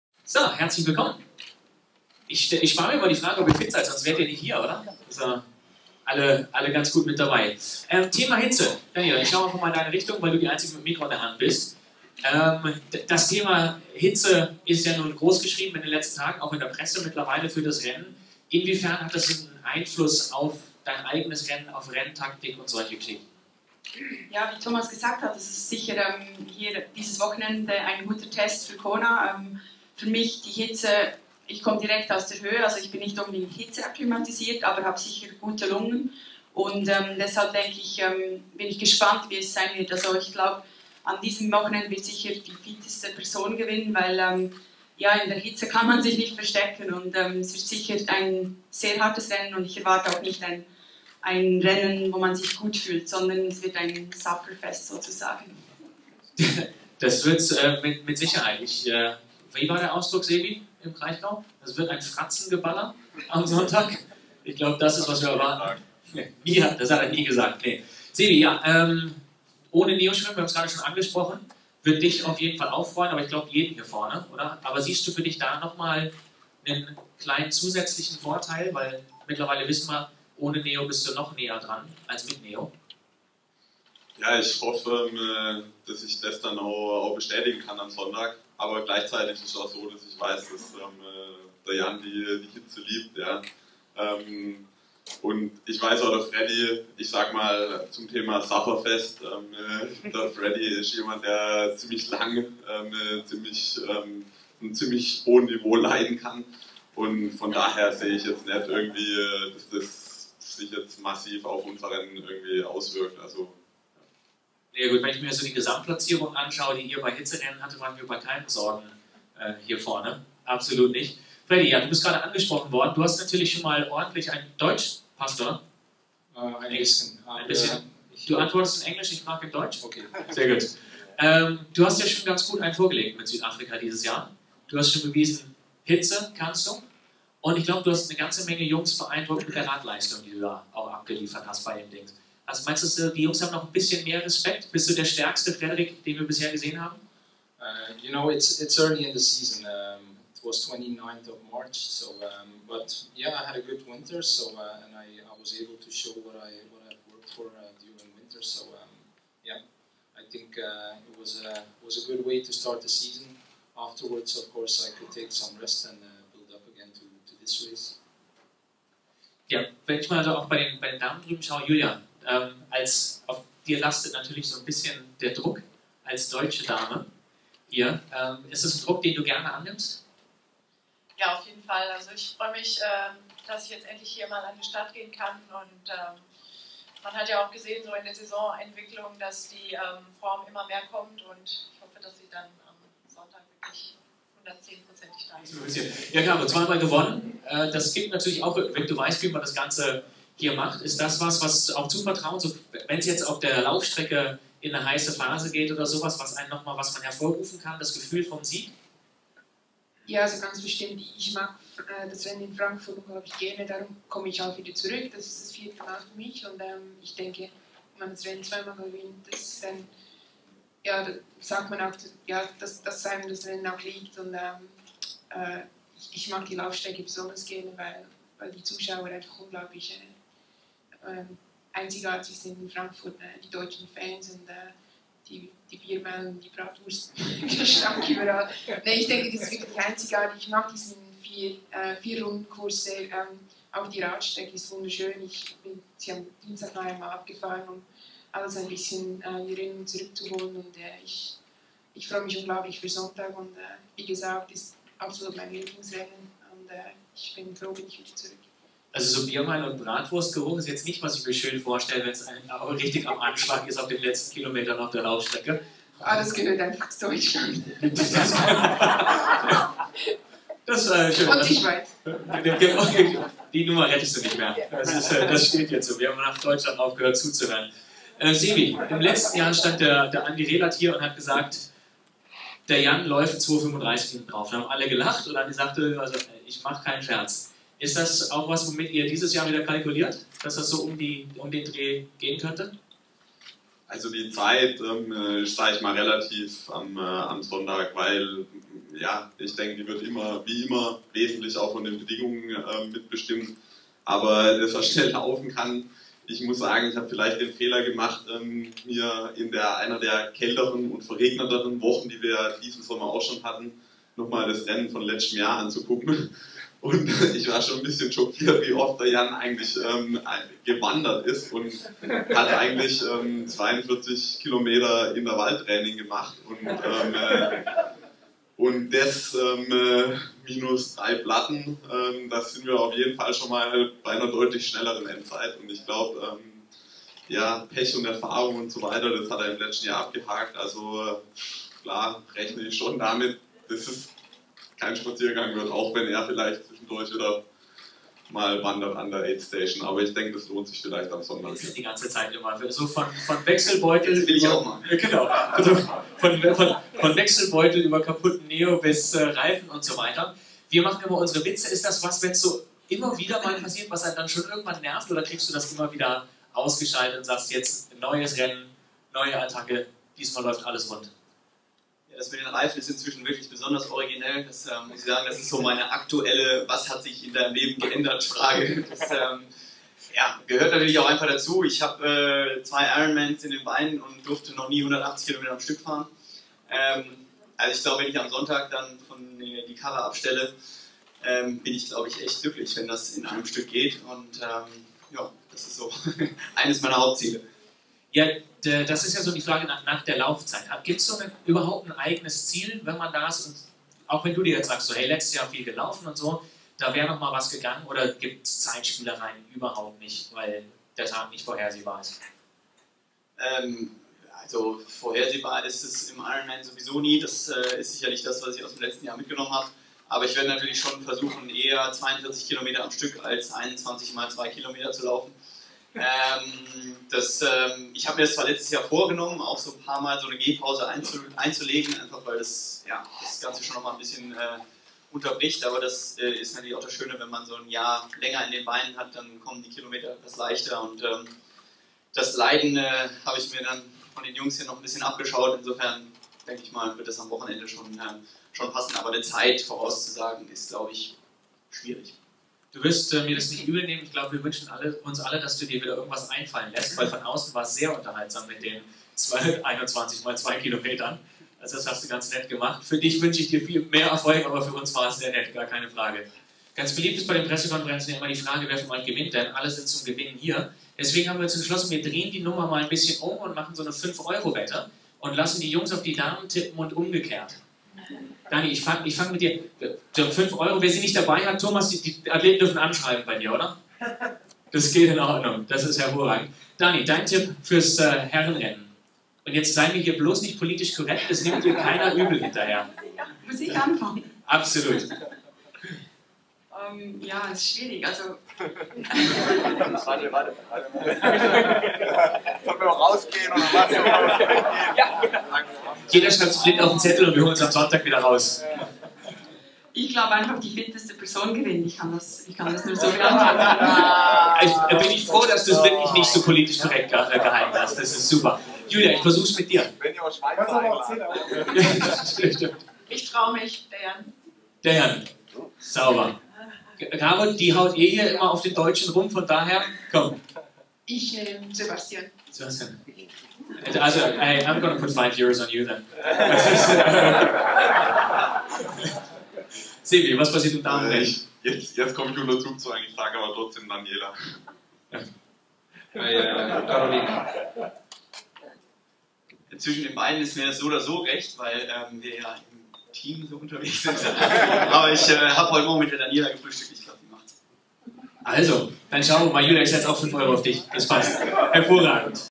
"Suffer-Fest" IRONMAN EM Pre-Race Pressekonferenz: Was sagen die Top Profis?
Bei der IRONMAN European Championship in Frankfurt wird ein "Suffer-Fest" erwartet. Das sagt zumindest Daniela Ryf mit Blick auf die vom Wetterbericht vorhergesagte Hitze am Sonntag. Die versammelte Konkurrenz – allen voran Olympiasieger Jan Frodeno und der ehemalige IRONMAN-Weltmeister Frederik van Lierde konnten nur zustimmend nicken. Es stand allen ins Gesicht geschrieben: Der Respekt vor den prognostizierten Temperaturen am Renntag ist auch bei den Profis groß. Athletes Mind Tuning war live dabei und hat ein paar Eindrücke von den Top Profis vor dem Rennen eingesammelt.

00:40 Min: Daniela Ryf (IRONMAN 70.3-Weltmeisterin) "Suffer-Fest": "Bei diesem Wetter kann man sich nicht verstecken. Es wird sicher ein sehr hartes Rennen und ich erwarte auch nicht ein Rennen wo man sich gut fühlt, sondern es wird ein Suffer-Fest sozusagen."

03:00 Min: Julia Gajer (Siegerin 70.3 Ironman in Luxembourg) - Zum Stichwort Druck: "Ich nehme den Druck auf jeden Fall gerne an. Ich freue mich auf den Start und hoffe, dass die Form dann am Sonntag 110% da ist."

05:05 Min: Sebastian Kienle (amtierender IRONMAN-Weltmeister) - Über Jan Frodeno (Olympiasieger): "Ich muss sagen ich habe vielleicht den Fehler gemacht, mir an einem der verregneten Tage der letzten Wochen das Video vom Rennen im letzten Jahr anzusehen. Und ich war schockiert zu sehen wie oft Jan da gewandert ist. Er hat eigentlich 42km Intervall-Training gemacht, minus 3 Platten - (Anmerkung AMT: wenn wir das ausrechnen) dann landen wir bei einer deutlich schnelleren Zeit. Ich rechne schon damit, dass es kein Spaziergang wird, auch wenn er vielleicht zwischendurch wieder Mal wandert an der Aid-Station"

06:12 Min: Jan Frodeno (Olympiasieger) - Zum Pech im letzten Jahr: "Das mit den Reifenpannen im letzten Jahr ist inzwischen so die 'was hat sich in deinem Leben geändert - Frage'. Das gehört dazu. Schließlich habe ich zwei Ironman Rennen in den Beinen und ich durfte noch nie 180km Rad am Stück fahren. Am Sonntag die 180km am Stück zu fahren ist eines meiner Hauptziele. "

07:50 Min: 5 EUR Wette auf den Sieger: Spannend wurde es bei einer kleinen Wette, zu der Moderator Till Schenk aufrief. Die Damen sollten bei den Herren auf den Sieger tippen und vice versa: Jan Frodeno und Sebastian Kienle einigten sich auf Daniela Ryf, van Lierde tippte auf Steffen. Bei den Damen sahen Steffen und Gajer Kienle als Sieger, Ryf ließ sich nicht zu einer Prognose hinreißen.

Es wird also sicher ein spannendes Rennen am Sonntag. Ich wünsche allen Startern bestmögliche Leistungen und einen fairen Wettkampf. Wenn du nicht selbst am Start bist, empfehle ich dir das Rennen live in Frankfurt anzuschauen oder über die Berichterstattung mitzuverfolgen. Nutze die Gelegenheit und studiere die besten Athleten. Du kannst sicher etwas lernen. Erfolgreiche Sportler zu beobachten ist auch eine Form von Mental Training. Ich bin auf jeden Fall gespannt, wer seine mentale Stärke in einen Sieg verwandeln kann.